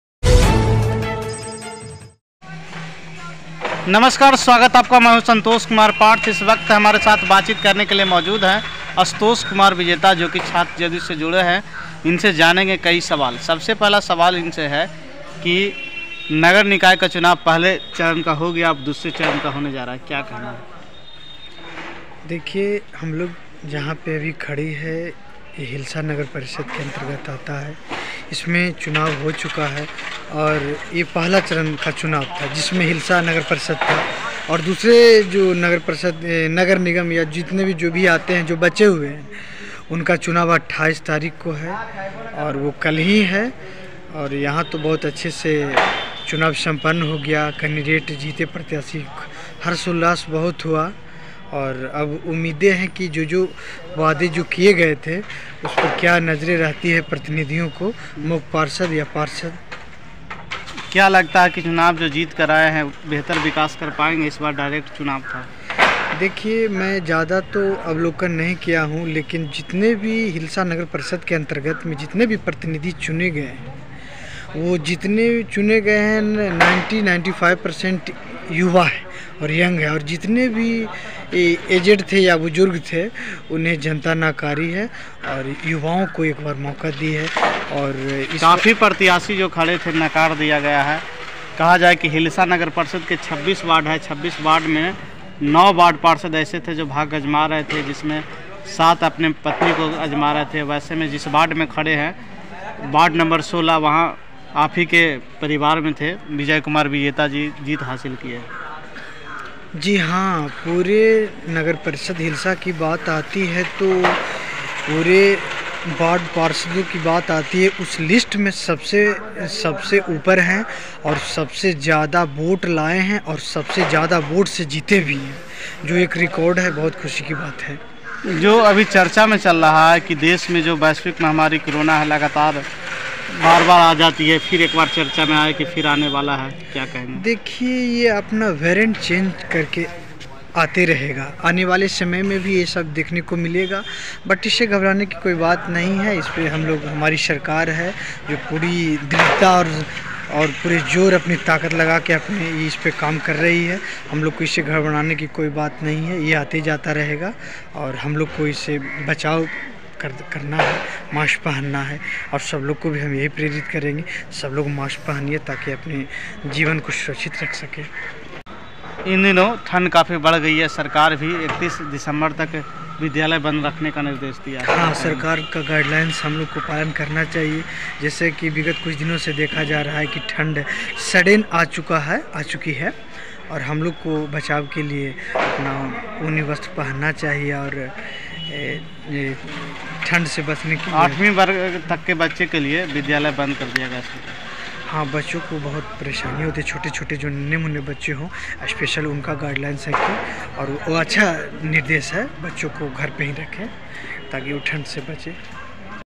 नगर परिषद से जुड़े सवाल को लेकर बातचीत आशुतोष कुमार विजेता से
नगर परिषद से जुड़े सवाल को लेकर
 बातचीत आशुतोष कुमार विजेता से